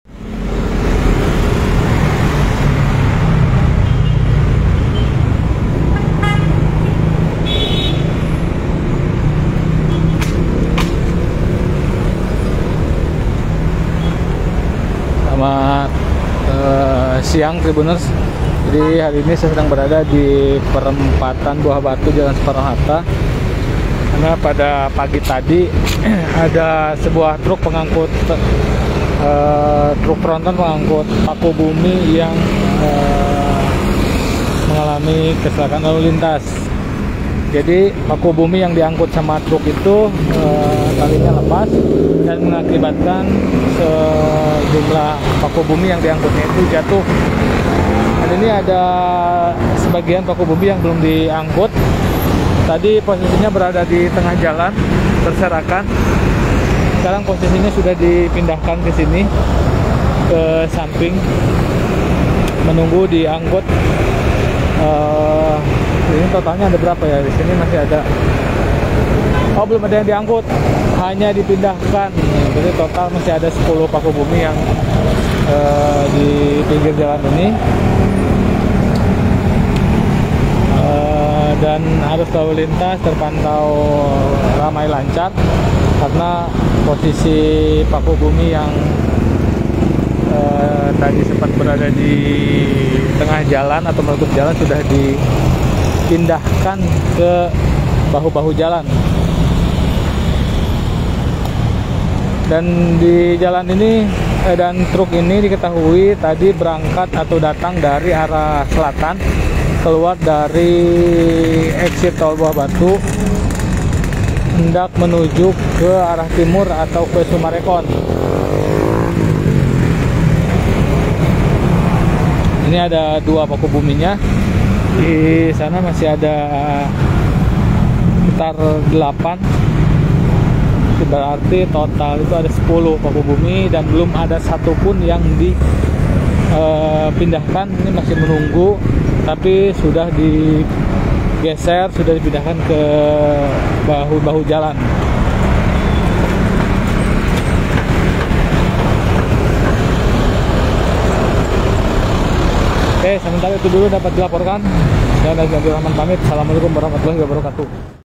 Paku Bumi Berjatuhan dari Truk Tronton, Saat Ini Masih Berada di Bahu Jalan
TRIBUNPRIANGAN.COM - Beberapa paku bumi dikabarkan jatuh dari truk tronton di Jalan Soekarno-Hatta, Kota Bandung, Jawa Barat pada Sabtu (2/11/2024).

Saat ini, paku bumi yang sempat berserakan di tengah jalan sudah diamankan ke bahu jalan. (*)

Laporan Nazmi Abdurrahman
Prod. Video: Aldi M Perdana